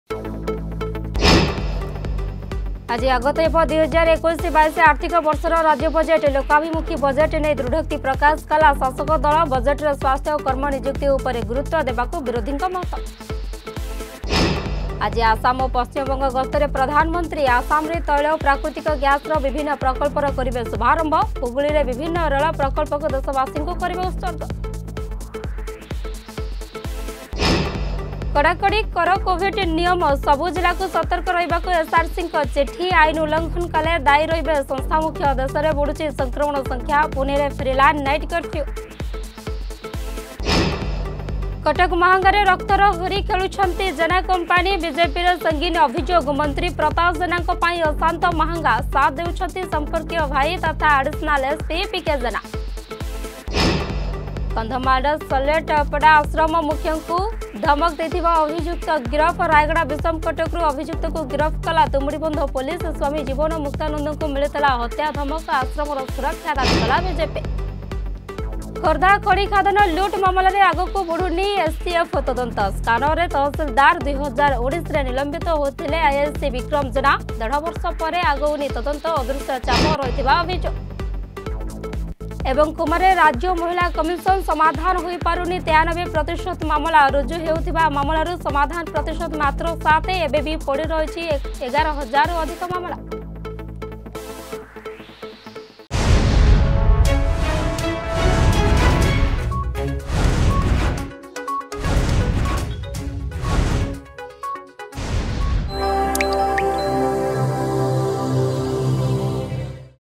Headlines 7Am
ଆଜି ଆଗତ ହେବ 2021-22 ଆର୍ଥିକ ବର୍ଷର ରାଜ୍ୟ ବଜେଟ...
ଆଜି ଆସାମ ଓ ପଶ୍ଚିମବଙ୍ଗ ଗସ୍ତରେ ପ୍ରଧାନମନ୍ତ୍ରୀ.
କଡ଼ାକଡ଼ି କର କୋଭିଡ ନିୟମ...
କଟକ ମାହାଙ୍ଗାରେ ରକ୍ତର ହୋରି ଖେଳୁଛନ୍ତି ଜେନା କମ୍ପାନୀ...
କନ୍ଧମାଳର ଜଲେସପଟ୍ଟା ଆଶ୍ରମ ମୁଖ୍ୟଙ୍କୁ ଧମକ ଦେଇଥିବା ଅଭିଯୁକ୍ତ ଗିରଫ...
ଖୋର୍ଦ୍ଧା ଖଣିଖାଦାନ ଲୁଟ ମାମଲାରେ ଆଗକୁ ବଢୁନି ଏସଟିଏଫ୍ ତଦନ୍ତ...
କୋମାରେ ରାଜ୍ୟ ମହିଳା କମିଶନ.